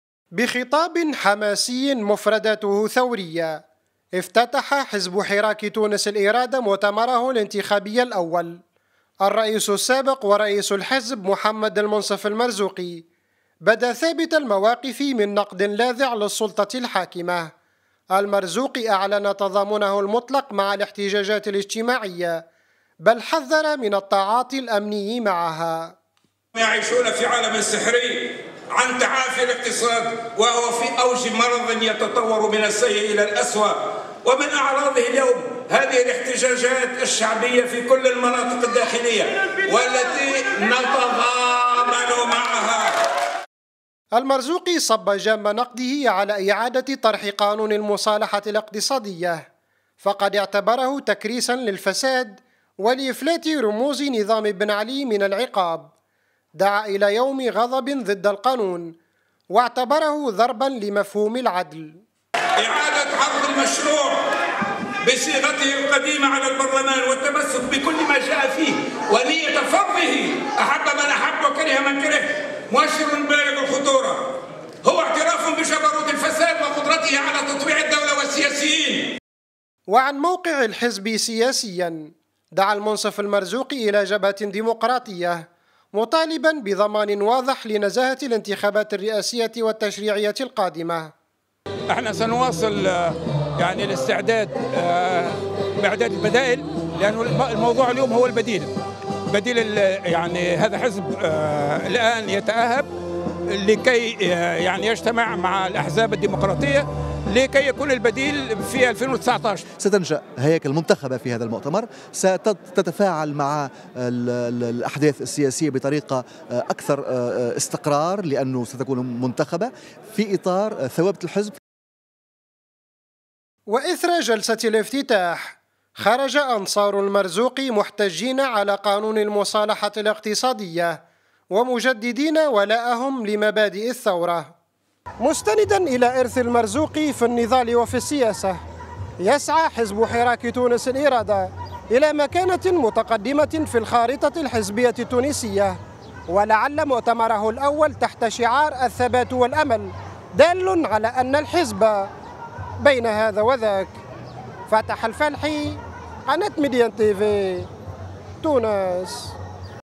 فيديو .. المرزوقي ينتقد الاتجاه الذي تسير فيه السلطة الحاكمة
اعتبر الرئيس التونسي السابق المنصف المرزوقي أن كل المؤشرات الاقتصادية والاجتماعية تدل على أن تونس، تسير في الاتجاه الخاطئ بسبب فشل السلطة الحاكمة في إدارة البلاد .

وأعلن المرزوقي في افتتاح المؤتمر الانتخابي الأول لحزبه تضامنه المطلق مع الاحتجاجات الاجتماعية بالبلاد محذرا من مغبة معالجتها أمنيا.

فاتح الفالحي